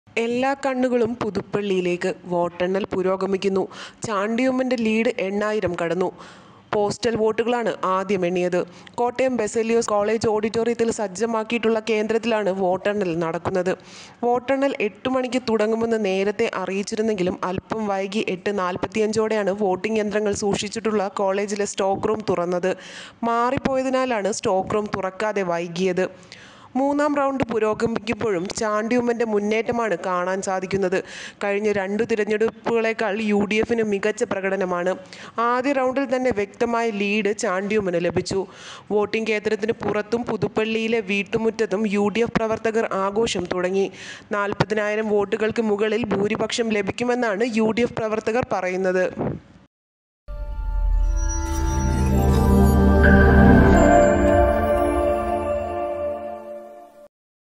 ചാണ്ടി ഉമ്മൻ അതിവേഗം ബഹുദൂരം , മൃഗീയ ഭൂരിപക്ഷം ലഭിക്കുമെന്ന് സൂചന
Tag-Generator-Youtube-malayalam news-global